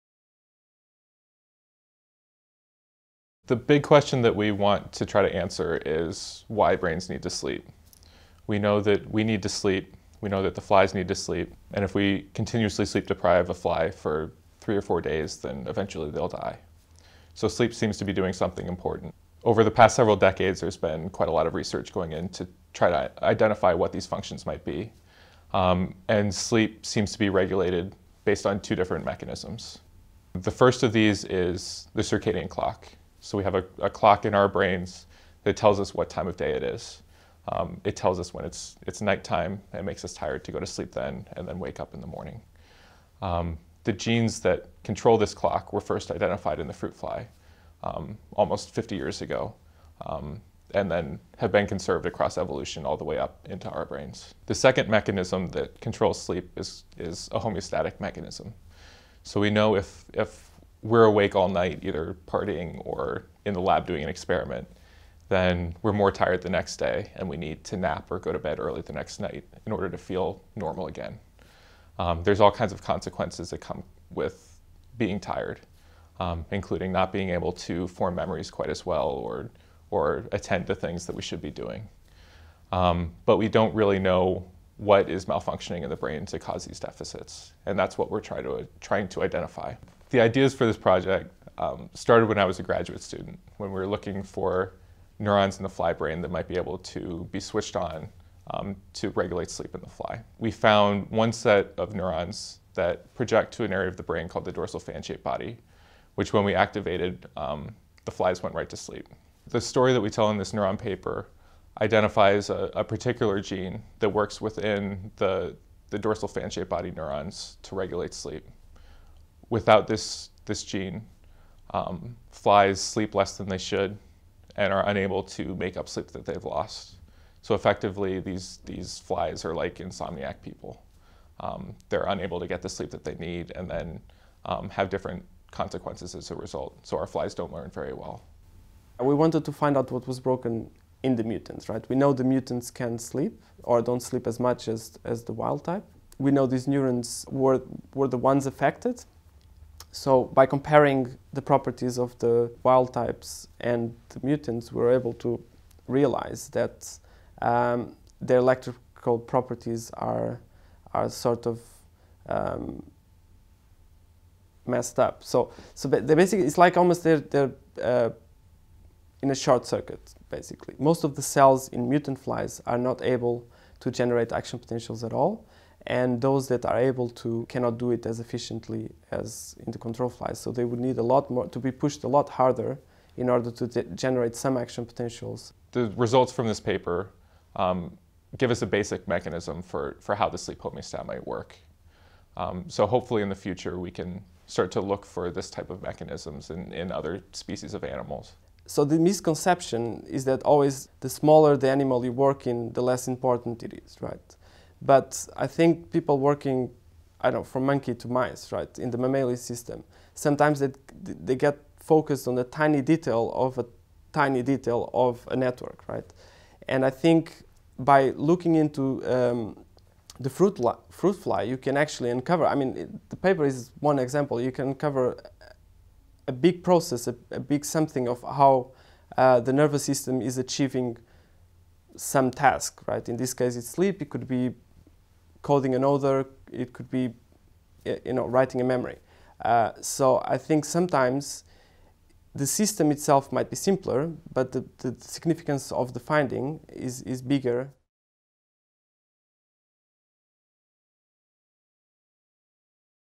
Why do brains need to sleep?
Jeff Donlea and Diogo Pimontel, postdocs at the Centre for Neural Circuits and Behaviour, discuss some of the genes that underpin our sleep cycle: and how experimental work is helping to understand the processes that make sleep so important.